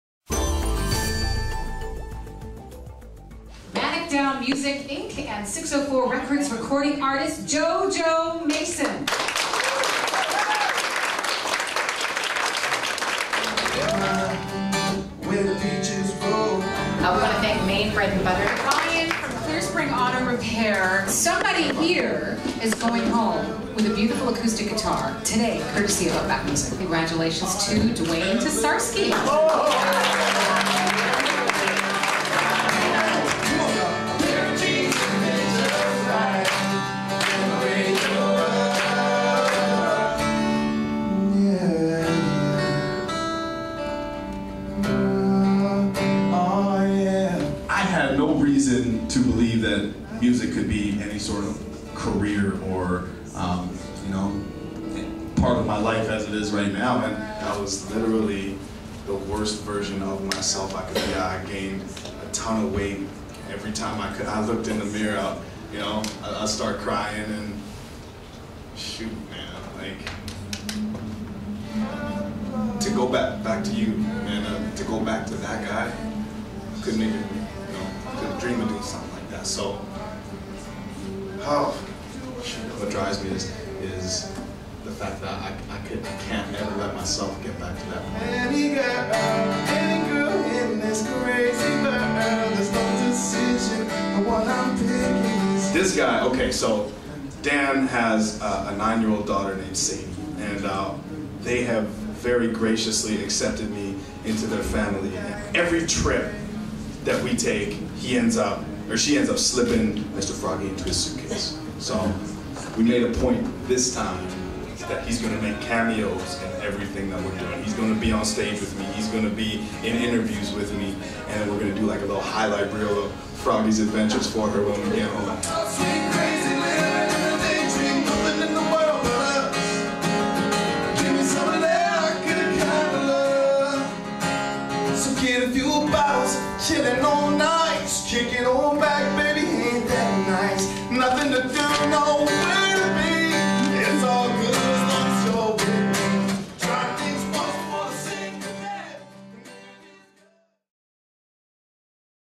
Live Lounge With Jojo Mason
Jojo Mason performs an acoustic set for the winners of Country 107's "Live Lounge" contest!